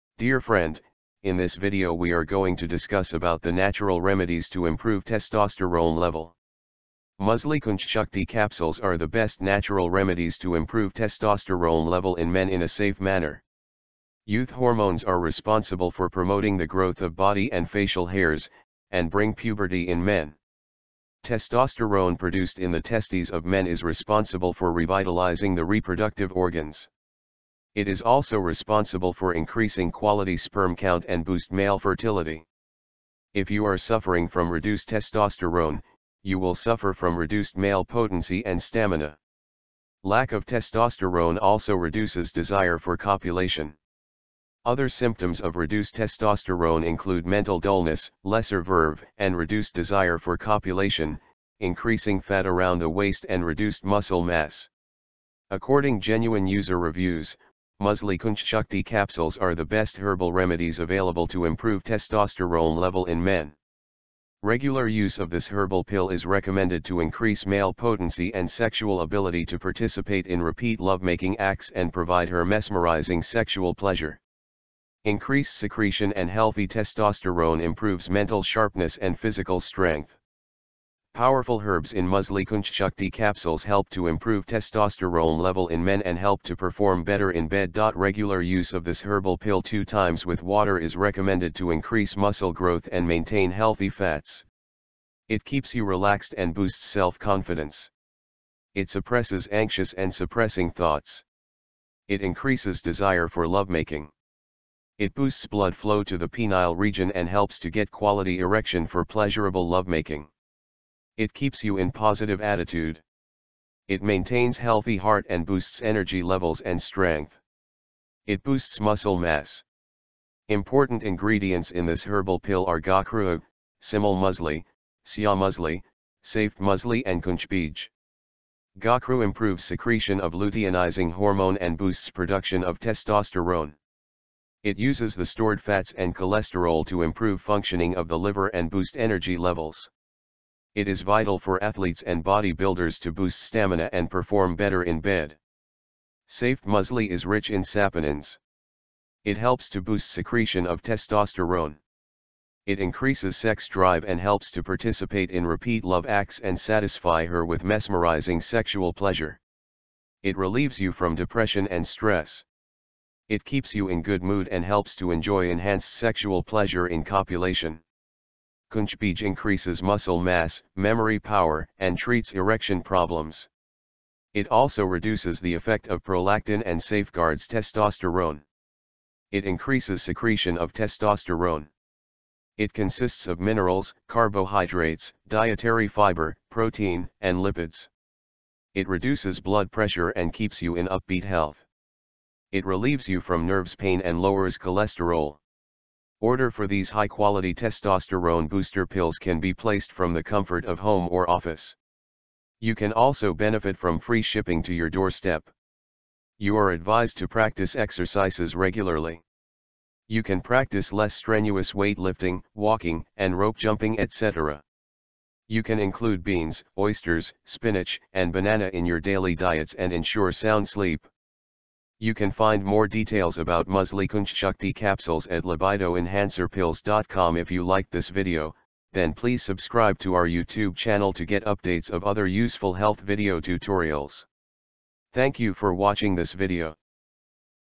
Natural Remedies To Improve Testosterone Level In Men
Natural Remedies To Improve Testosterone Level

Dear friend, in this video we are going to discuss about the natural remedies to improve testosterone level. Musli Kaunch Shakti capsules are the best natural remedies to improve testosterone level in men in a safe manner.